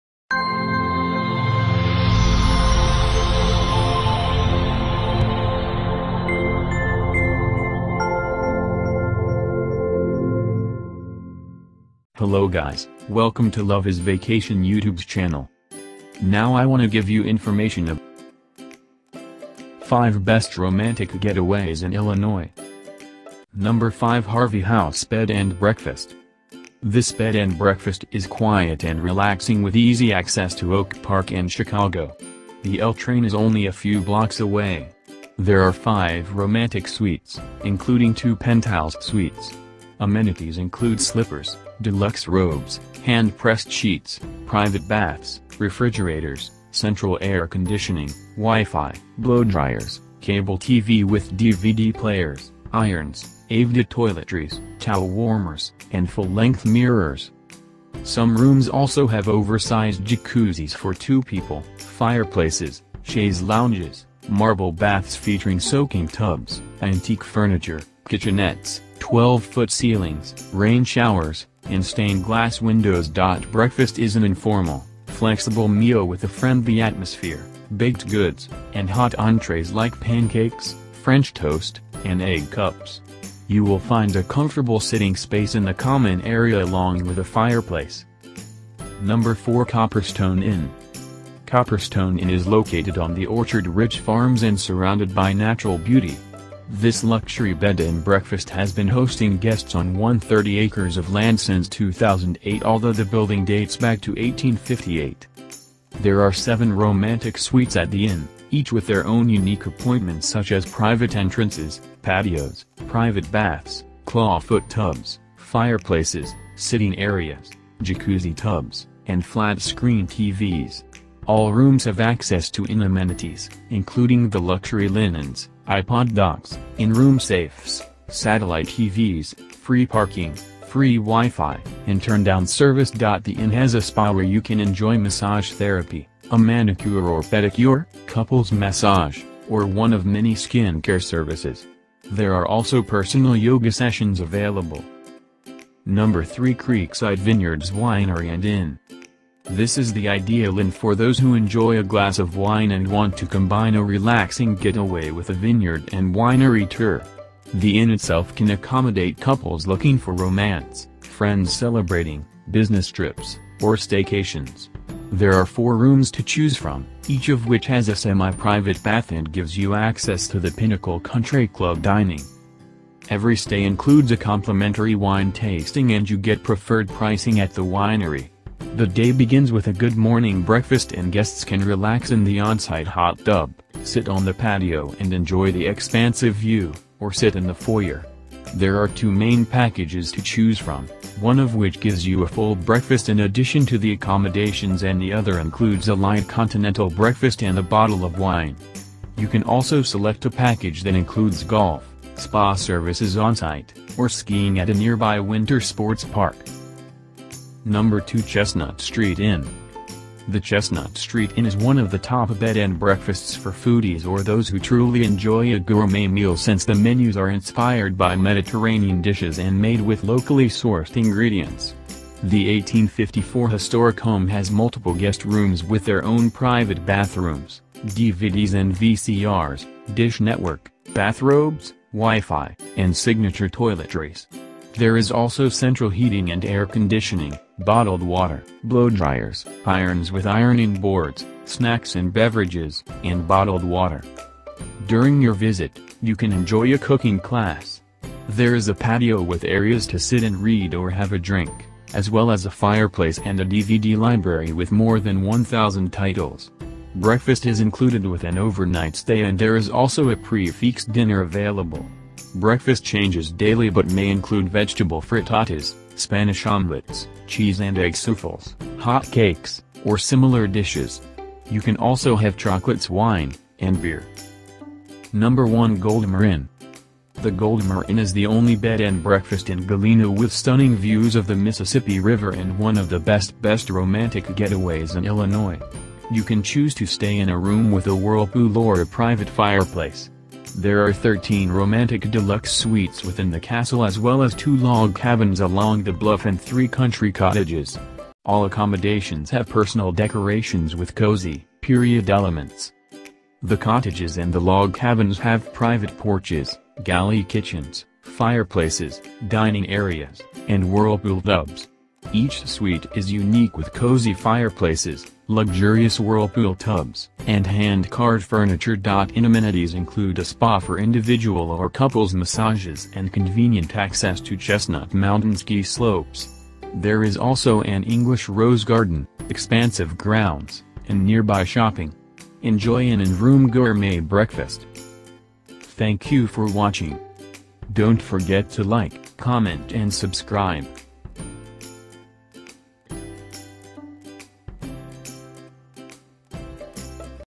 5 Best Romantic Getaways in Illinois | Love is Vacation
5 Best Romantic Getaways in Illinois

Plan a weekend trip to one of these romantic Illinois inns to relax and rejuvenate surrounded by nature. Whether you are looking for a spa getaway, wine tasting vacation, honeymoon suites, or ideas for foodies, there are many beautiful places to choose from, some of which are just a short drive from Chicago. Many of the inns that made our list have a unique history and all are known for their welcoming ambiance and outstanding service.

1 Goldmoor Inn
2 Chestnut Street Inn
3 Creekside Vineyards Winery & Inn
4 Copperstone Inn
5 Harvey House Bed and Breakfast